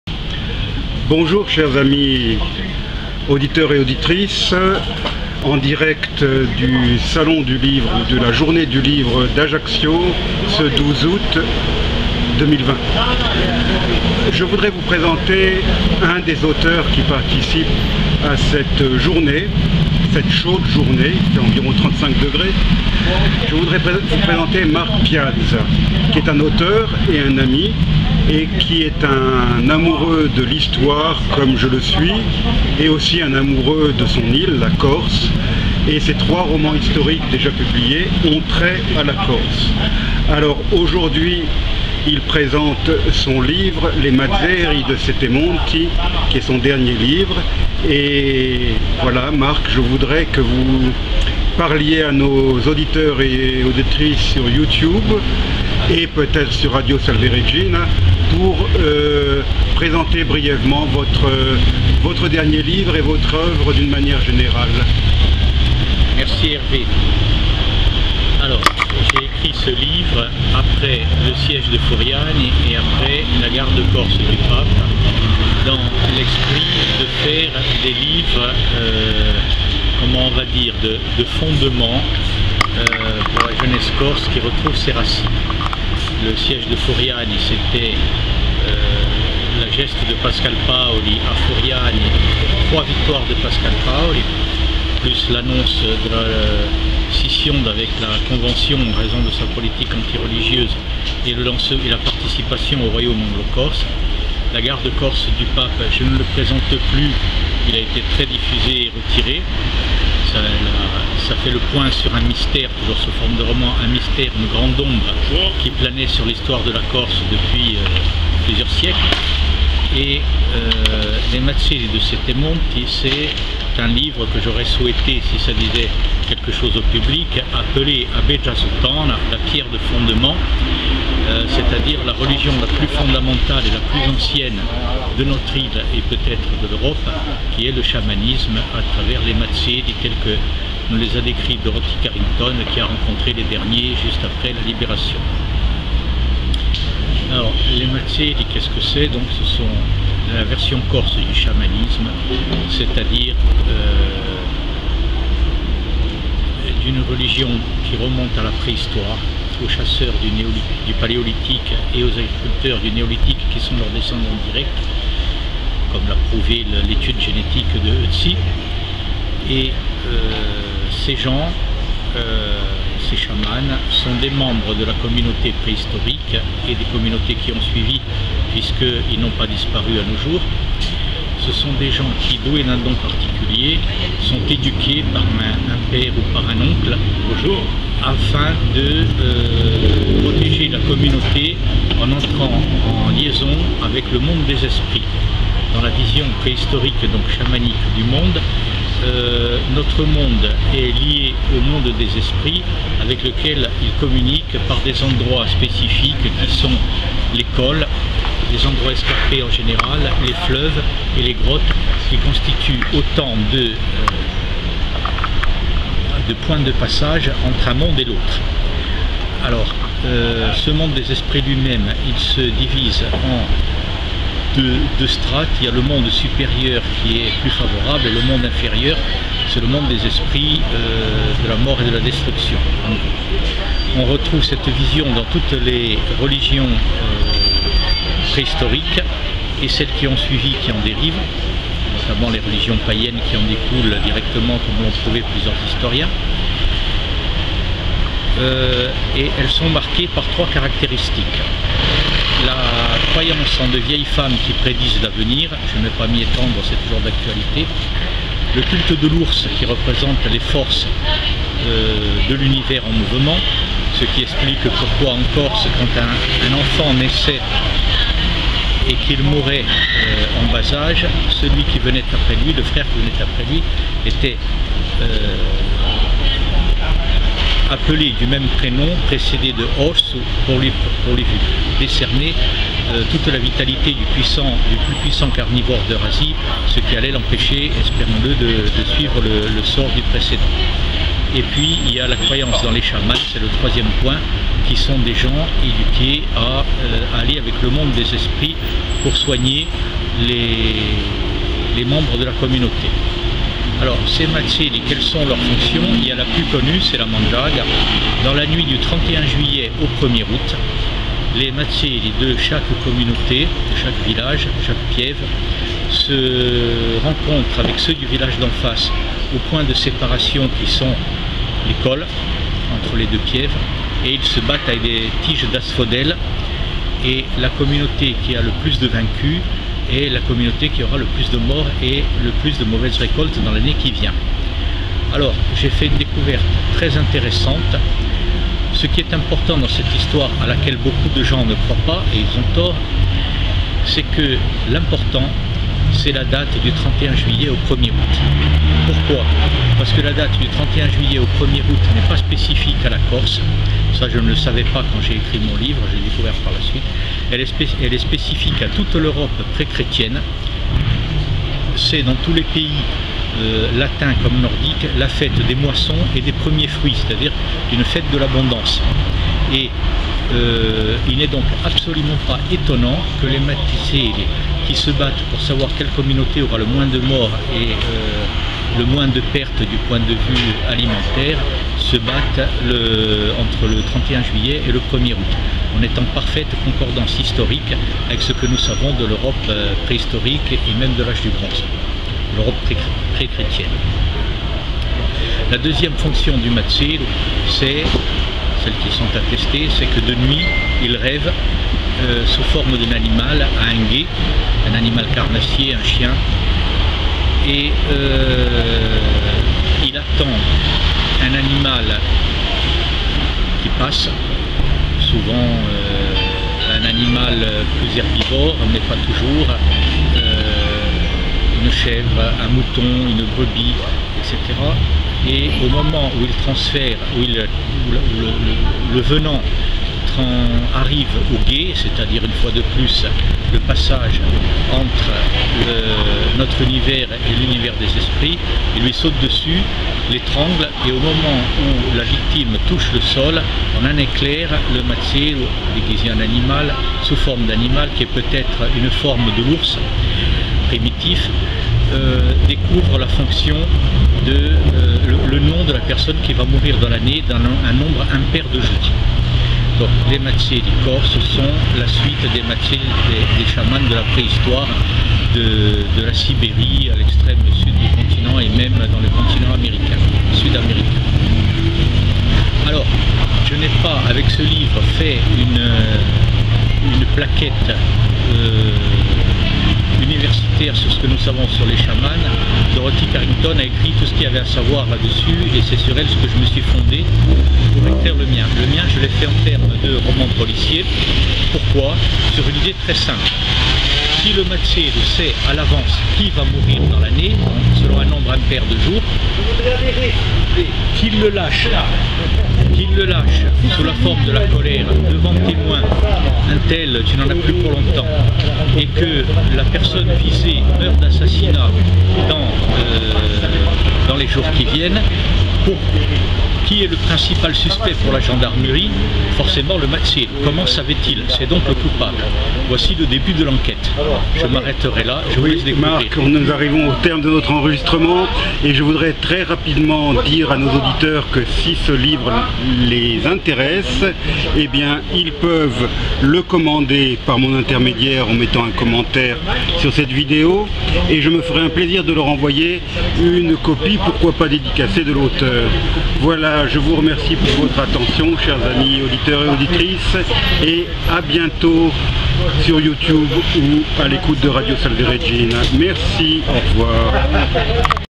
HervéTV n° 45: rencontre avec Marc Piazza, romancier corse
Marc Piazza est l'auteur de trois beaux romans historiques: "Le Siège de Furiani" (2012), "L'Affaire de la garde corse du Pape" (2015) et "Les Mazzeri de Settemonti" (2017). C'est de ce dernier ouvrage dont il est question dans l'entretien que cet Auteur amoureux d'Histoire nous a accordé place des Palmiers, à Ajaccio, lors de la Journée du Livre" du 12 août 2020.

Les lecteurs intéressés par les "Mazzeri de Settemonti" peuvent me contacter, nous pourrons leur faire parvenir cet ouvrage qui coûte 19,50 €.